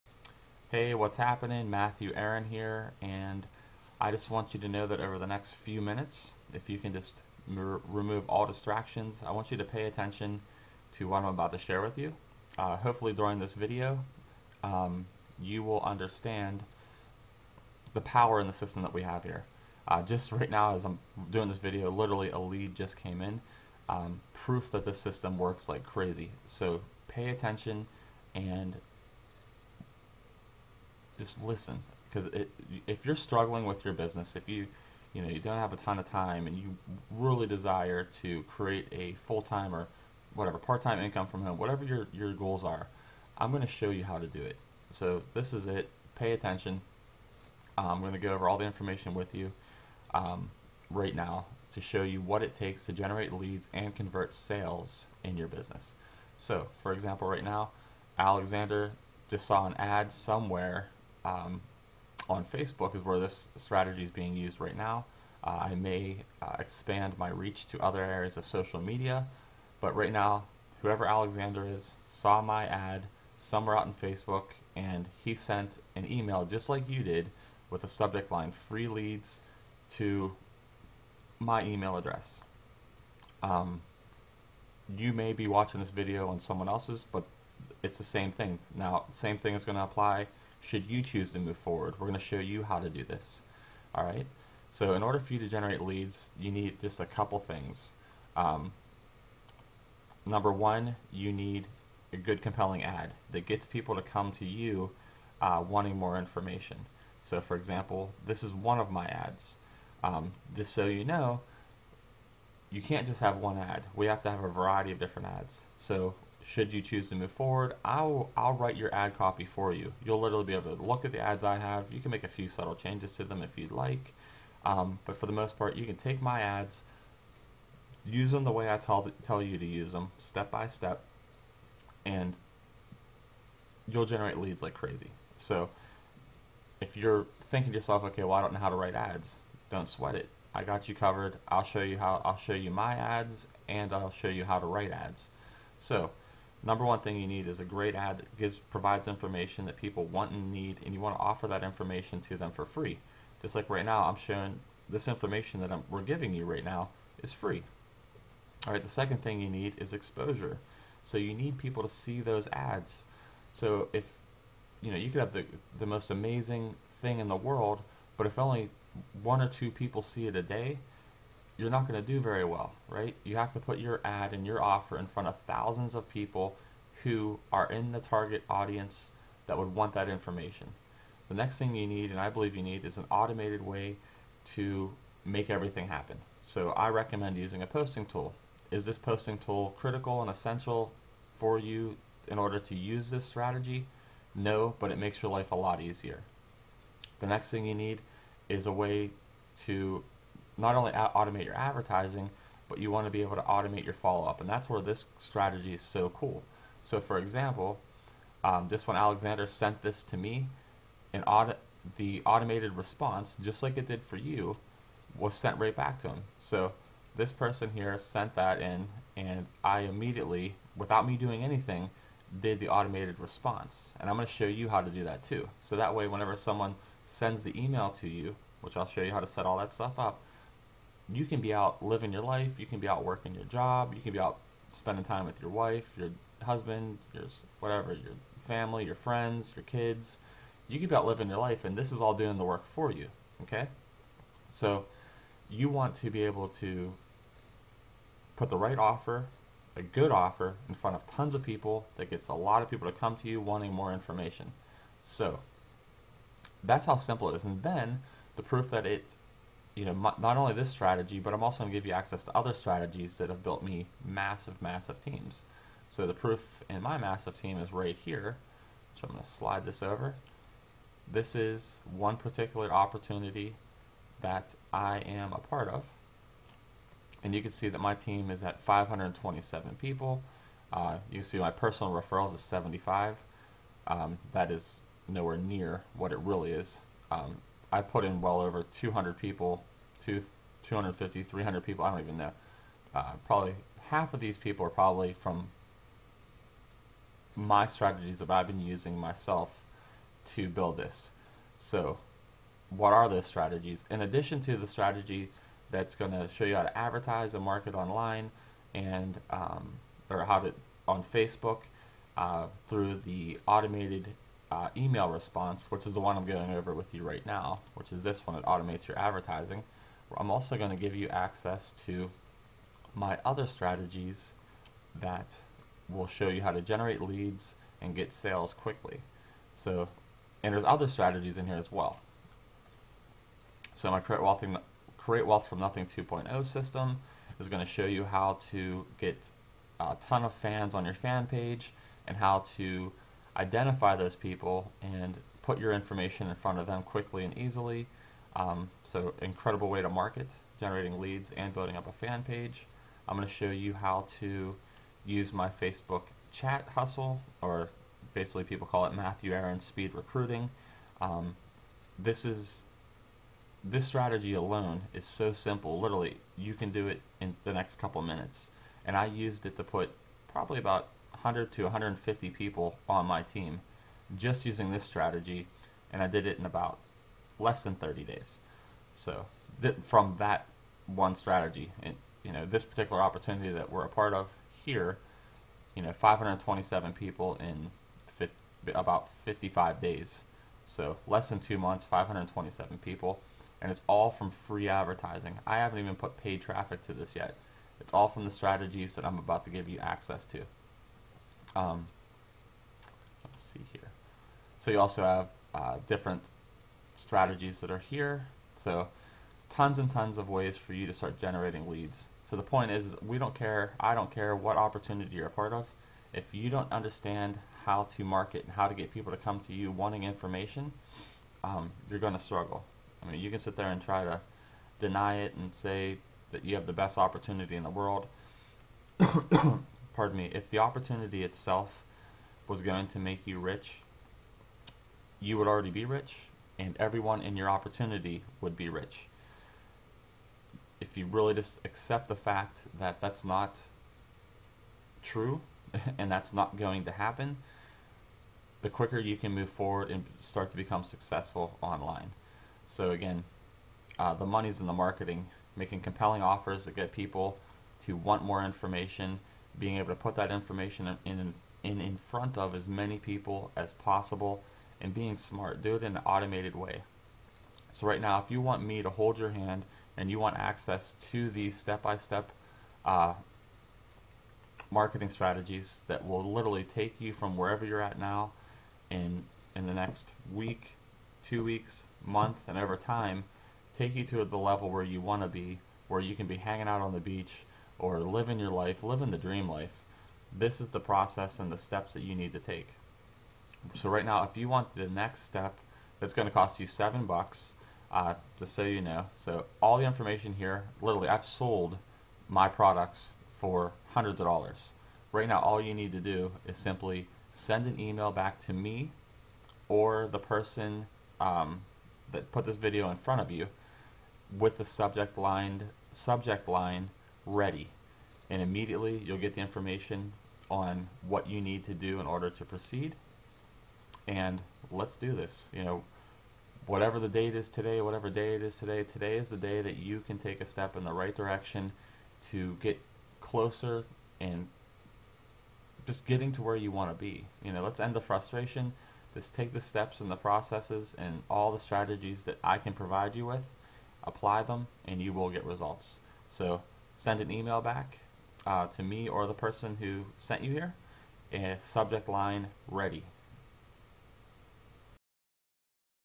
Generate Leads with Facebook and Gmail
This Quick Video Goes Over How To Generate Leads with Facebook and Gmail in an Automated Way.  For Full Access Send an email with the subject "Ready" to the person who you got this from or to freshnew